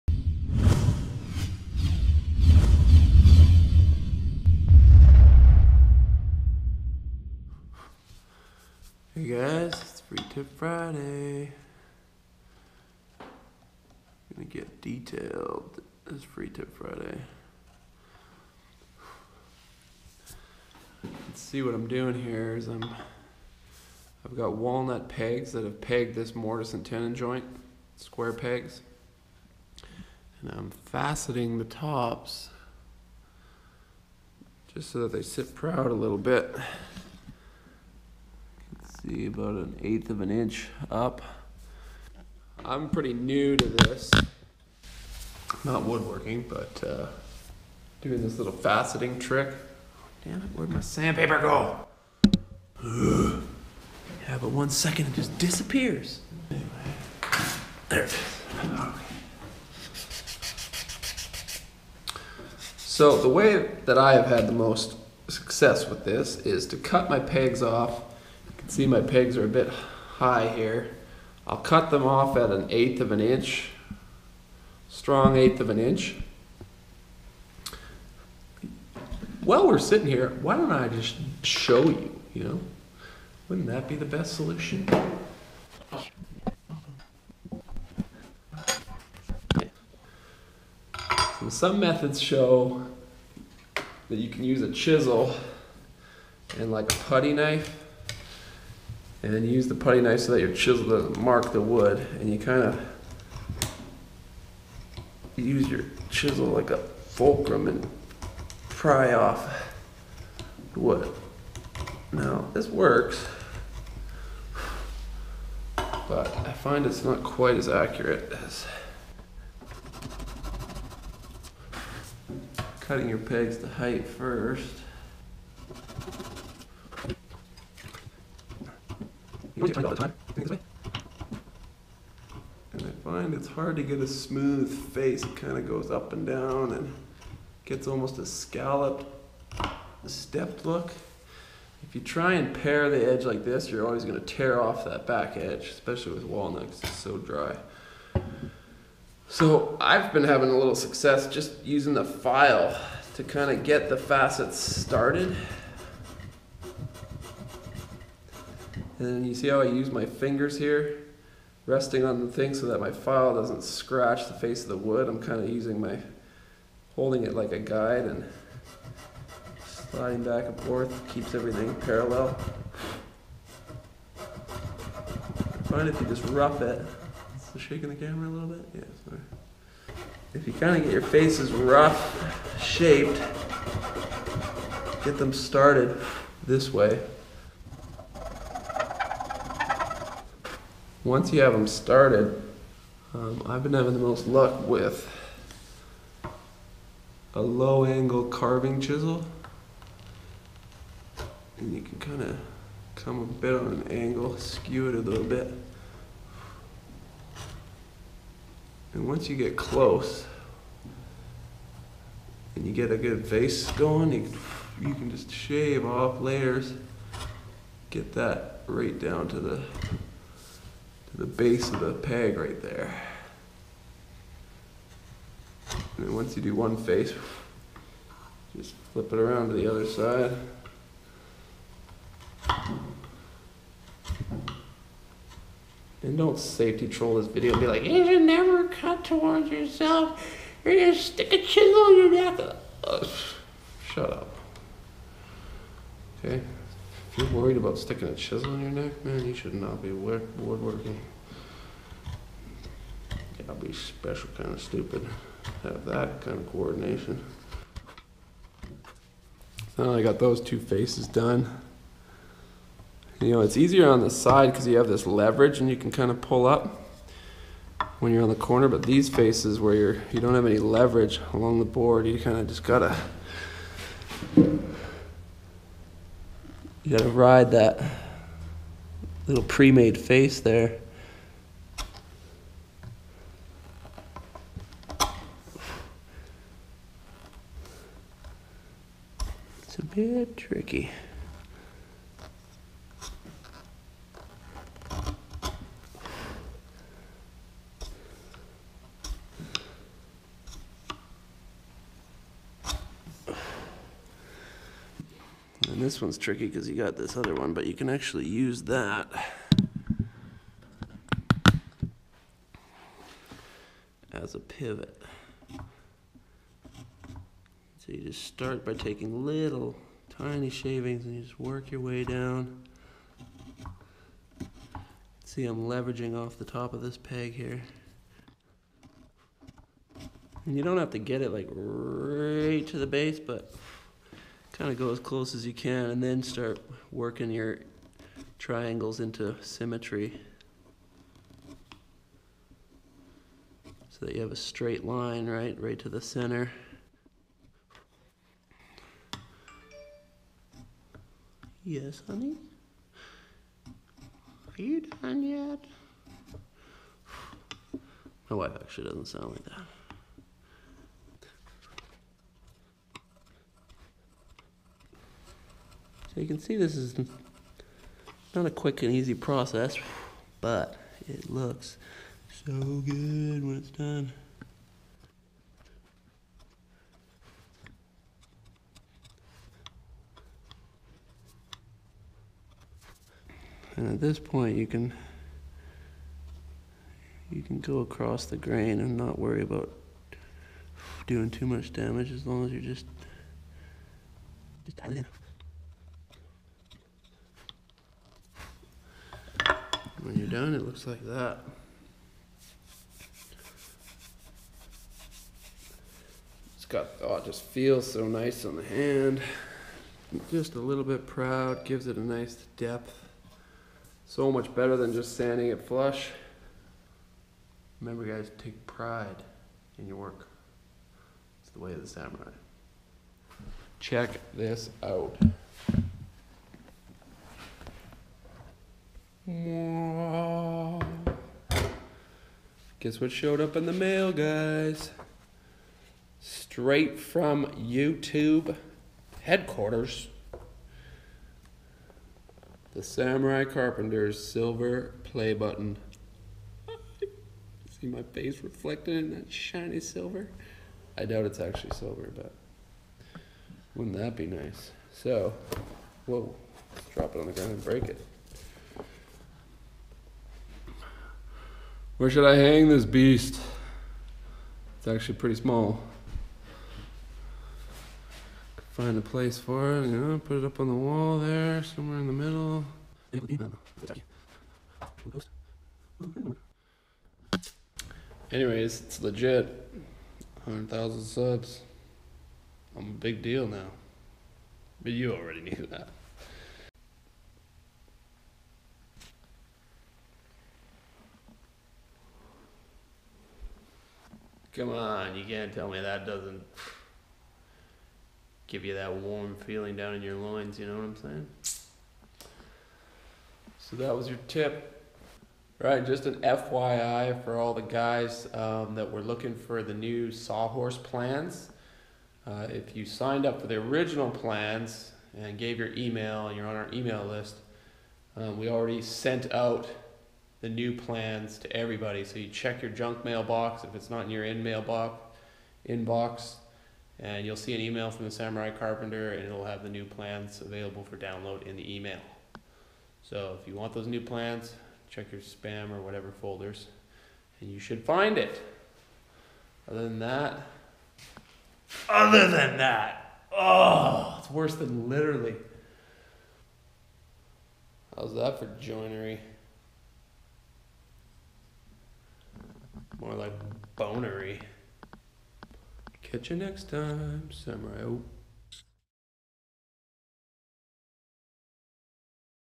FTF #12 Finishing A Pegged Mortise, How To
This is how I chisel a tapered point on square wood pegs to give them more sex appeal. It takes time but looks so good when it's done.